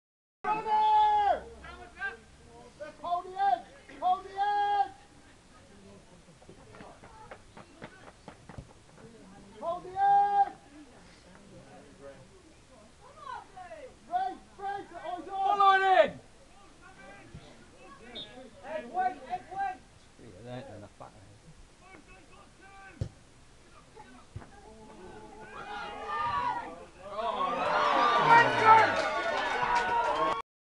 Duchy Premier League Game St Stephen 3 - 3 St Minver
Dale Beesley makes it 3 - 3 on 85 minutes for St Minver.